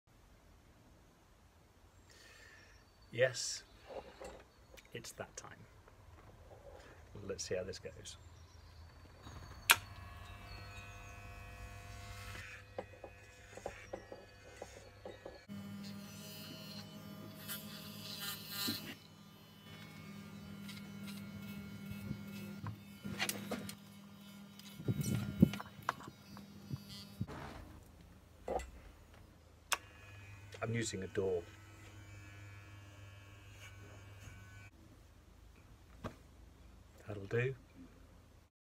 Lockdown self-haircut
Cutting my own hair for the first time ever.